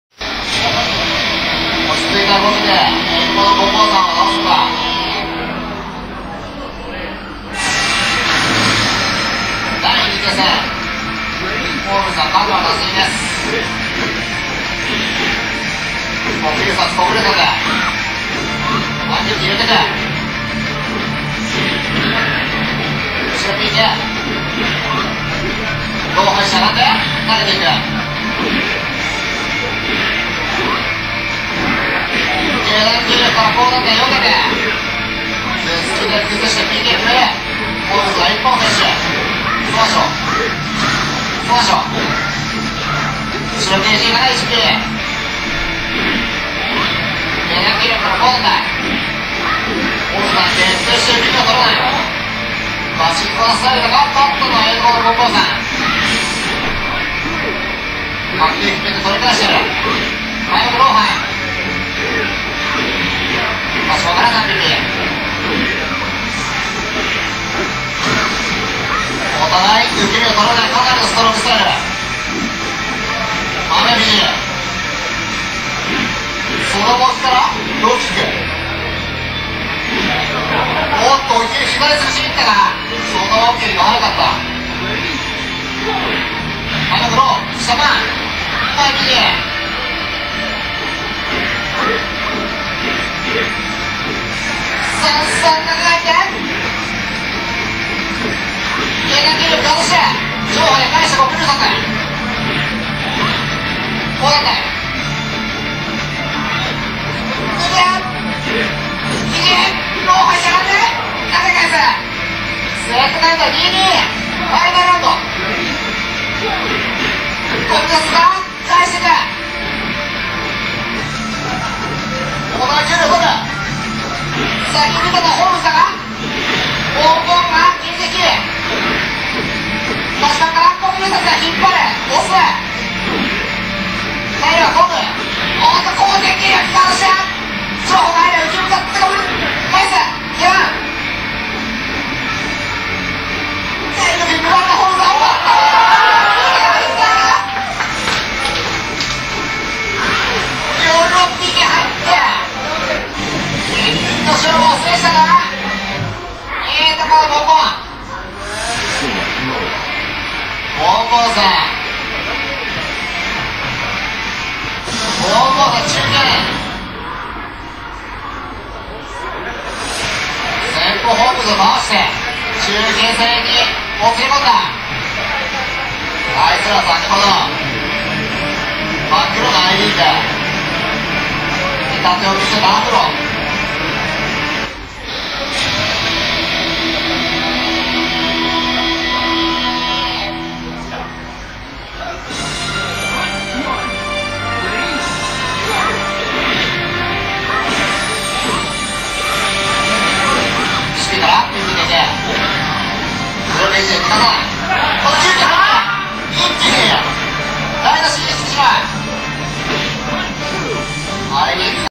ええとこのボンボン(PA) x ホームズ(GO)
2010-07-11大山ニュートンにて行われた第1回　称号Aランダム3on3大会の７/１１大山ニュートンランダム３オン３対戦動画です。